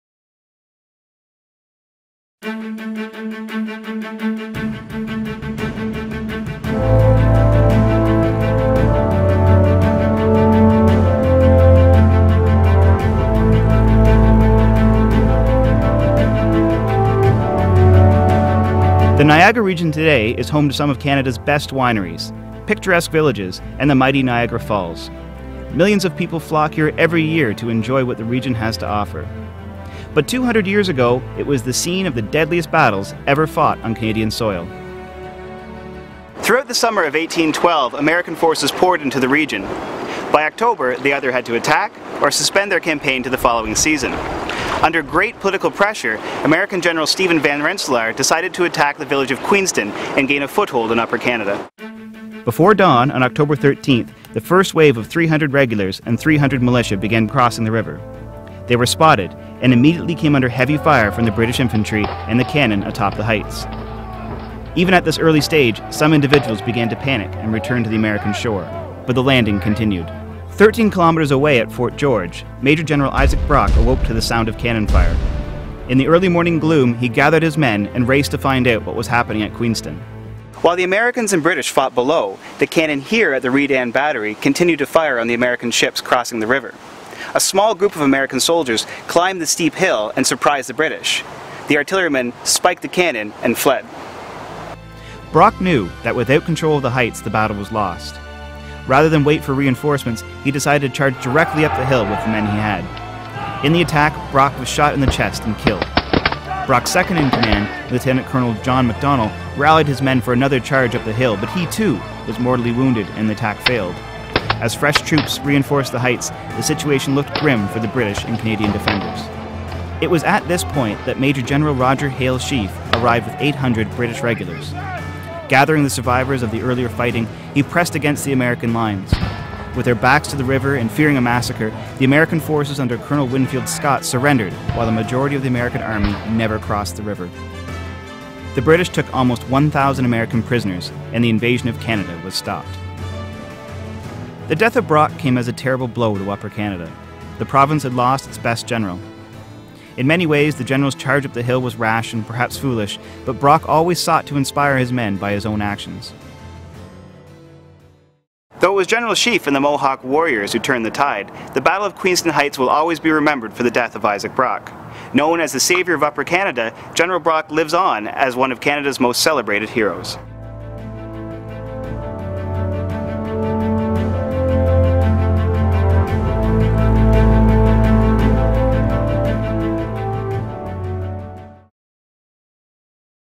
The Battle of Queenston Heights (October 1812)
Before dawn on October 13th, 300 American soldiers and 300 militia members led by famous American Colonel Winfield Scott crossed the Niagara River and into Canada. From there, they landed their boats and climbed up the steep, dangerous escarpment to get to the British camp at Queenston Heights. At first, the American attack was going well. Scott managed to capture the Redan battery at the top of the hill. This allowed him to take the British cannons — but the British weren’t easily defeated. And even though the Battle of Queenston Heights was a British victory, it will forever be known as the battle where Sir Isaac Brock was killed.  [This video was originally produced in 2012]

Join Tim Compeau in our series The War of 1812 - A Video Field Guide, as he brings to life the major events of this conflict.